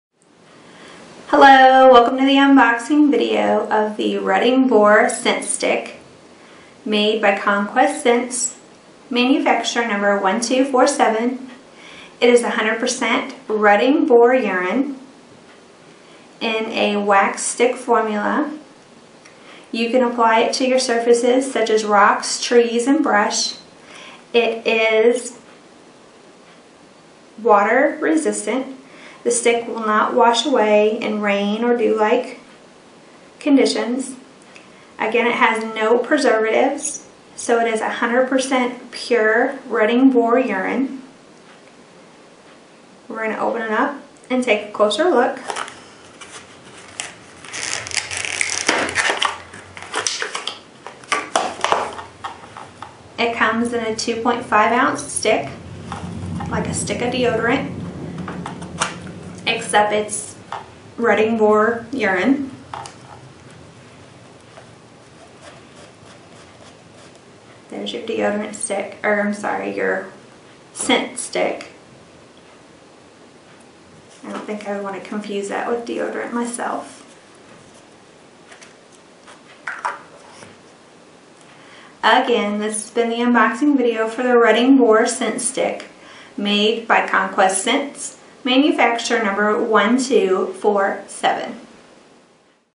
Conquest Sents Rutting Boar Scent Stick mfg# 1247
Conquest Sents Rutting Boar Scent Stick mfg# 1247, unboxing video. At Wholesale Hunter